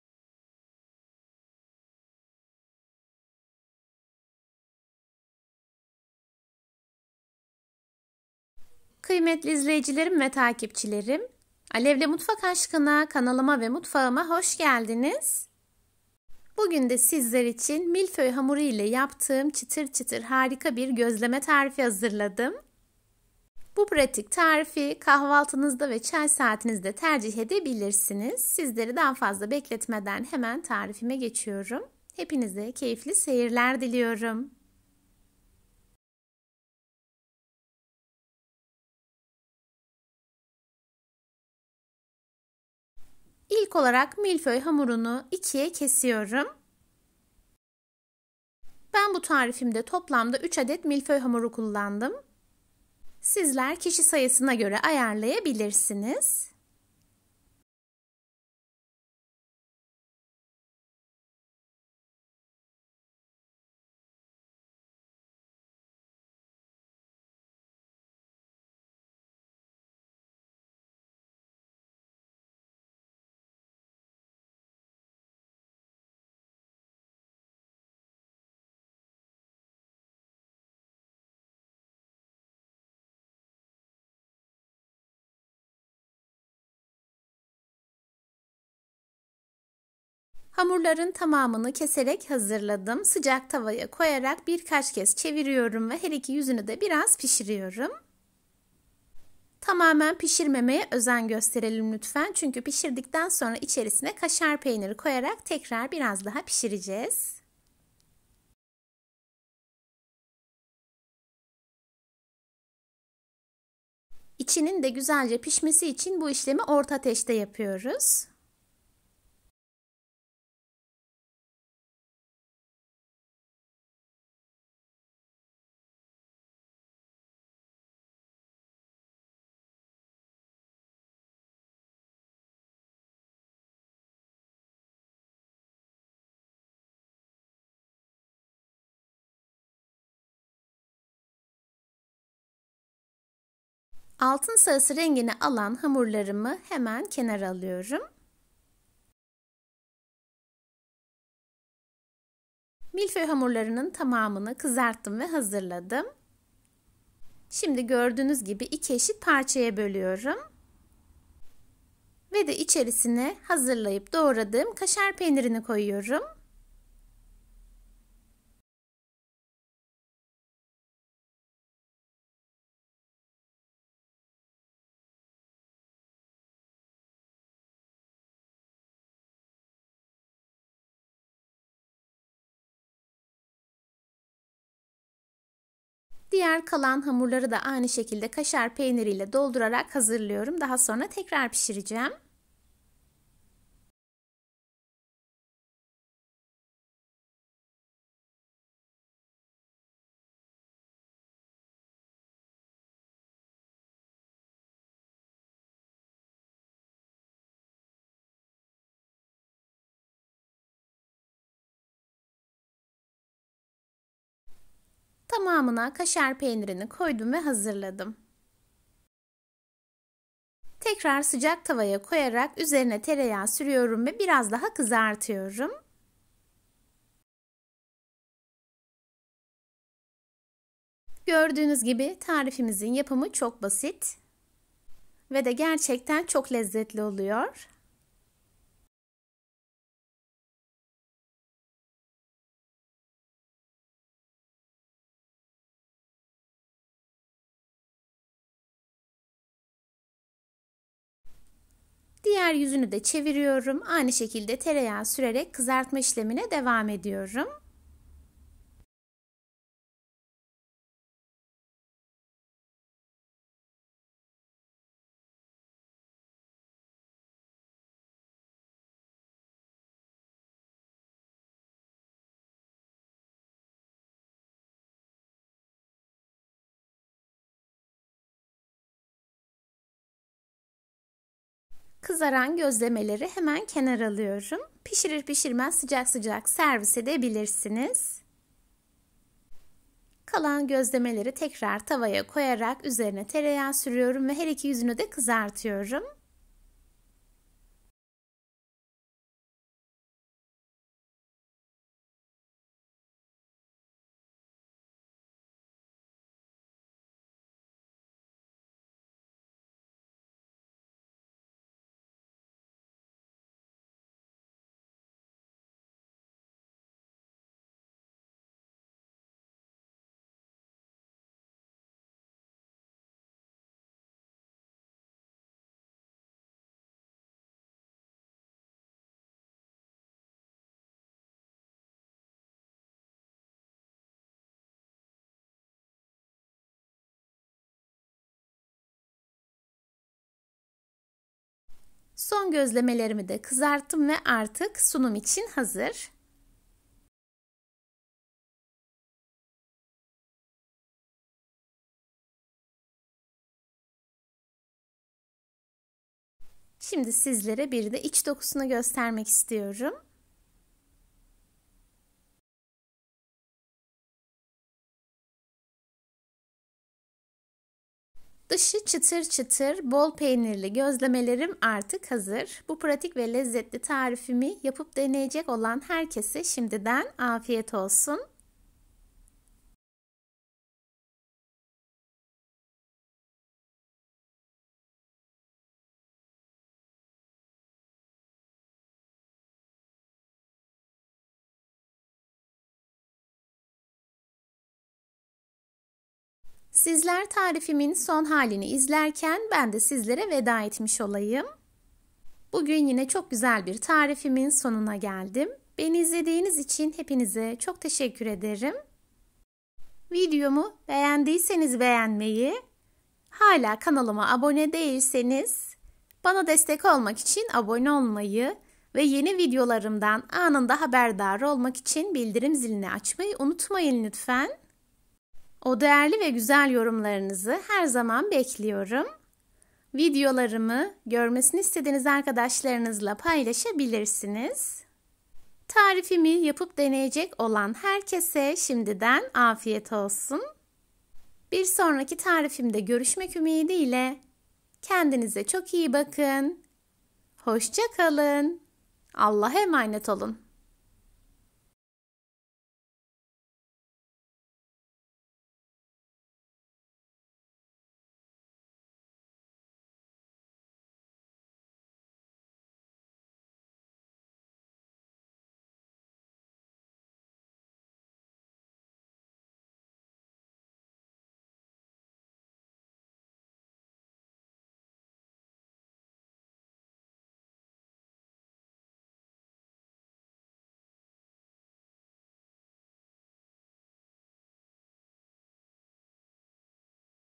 BAYRAM SABAHINA BU GÖZLEMELERİ MUTLAKA YAPIN 😋 EN KOLAY GÖZLEME TARİFİ / KAHVALTILIK
Kanalımdan ve mutfağımdan hepinize kucak dolusu sevgiler selamlar değerli izleyicilerim   bugün de sizler için nefis mi nefis bir gözleme tarifi hazırladım !! hamur yoğurmak yok hamur açmak yok  milföy hamuru ile yaptığım bu gözlemeler bir harika kahvaltıya ve çay saatine pratik bir şekilde yapabilirsiniz tarifimi yapıp deneyecek olan herkese şimdiden afiyet olsun
İYİ SEYİRLER 

Pişirme önerisi: orta ateş

Malzemeler
3 adet milföy hamuru ( kişi sayısına göre belirleyebilirsiniz )
Kaşar peyniri
Tereyağı

ENGLISH RECIPE

Cooking suggestion: medium heat

Materials
3 puff pastry dough (you can determine according to the number of people)
Cheddar cheese
Butter

. DEUTSCHES REZEPT

Kochvorschlag: mittlere Hitze

Materialien
3 Blätterteig (kann man je nach Personenzahl bestimmen)
Cheddar-Käse
Butter

. RECETA ESPAÑOLA

Sugerencia de cocción: fuego medio

Materiales
3 masas de hojaldre (podéis determinar según el número de personas)
Queso cheddar
Manteca

. RECEITA PORTUGUESA

Sugestão de cozimento: fogo médio

Materiais
3 massa folhada (você pode determinar de acordo com o número de pessoas)
Queijo cheddar
Manteiga

. РУССКИЙ РЕЦЕПТ

Рекомендации по приготовлению: средний огонь.

Материалы
3 слоеного теста (можно определить по количеству человек)
Сыр чеддар
Масло

. الوصفة العربية

اقتراح الطبخ: حرارة متوسطة

مواد
3 حبات من عجينة البف باستري (يمكنك تحديدها حسب عدد الأشخاص)
جبنة الشيدر
سمنة

. RECETTE FRANÇAISE

Suggestion de cuisson : feu moyen

Matériaux
3 pâtes feuilletées (à déterminer selon le nombre de personnes)
Fromage cheddar
Beurre